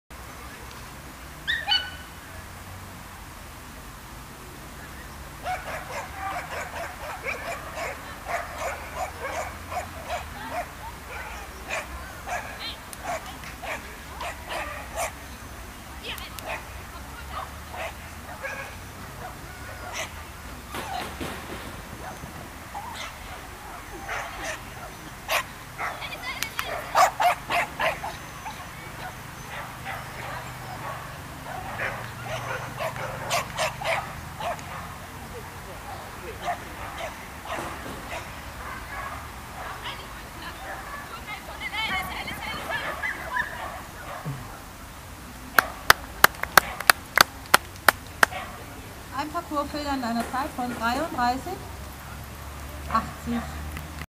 8. Sporthund-Cup in Heilbronn am 23.10.2011 Juliane Böttger & Alice Spiel mini.MP4
2. Platz  1 VW  Zeit  33,80 s  3,64 m/s
Richter: P.Stöpler  Sollzeit  37 s  Länge  123 m    4 Starter
Juliane & Alice  Spiel mini   SV OG Heilbronn